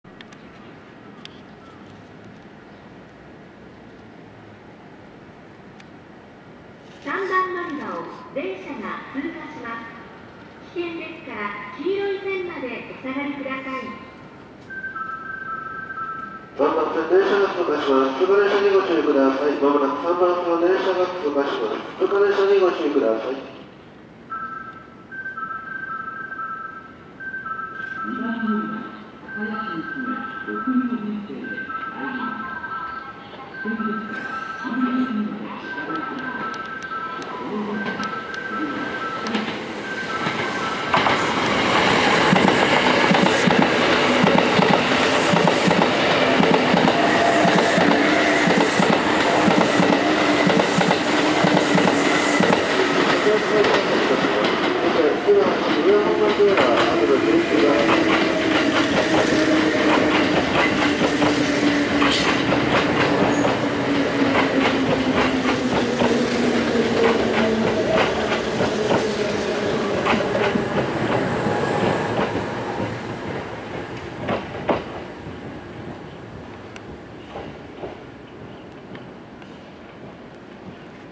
五十鈴川からの長距離快急!! 近鉄2430系AG43編成ほか10両(4+2+4) 快速急行大阪上本町行き 近鉄大阪線河内山本駅 通過
撮影日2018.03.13 近鉄大阪線 河内山本駅にて
2016年3月変更ダイヤ ※手持ち撮影です

快速急行 大阪上本町 3522ㇾ
近鉄2430系(AG43)+2410系(W21)+2800系(AX17)

[ダイヤ]
五十鈴川5:57発→宇治山田5:59着/6:01発→伊勢市6:02着/6:03発→松阪6:17着/発→伊勢中川6:24着/6:30発→榊原温泉口6:39着/6:40発→青山町6:53着/6:54発→名張7:08着/7:12発→榛原7:27着/7:28発→桜井7:36着/7:37発→大和八木7:42着/7:44発→大和高田7:49着/7:50発→五位堂7:53着/7:54発→鶴橋8:16着/発→大阪上本町8:19着

五十鈴川から約140km走行し来阪する２本目の快速急行です。

普通 高安 783ㇾ
近鉄2430系(G39)+(G36)

[ダイヤ]
大阪上本町7:42発→鶴橋7:44着/発→布施7:48着/7:50発→弥刀7:56着/8:00発→近鉄八尾8:04着/発→河内山本8:06着/8:07発→高安8:09着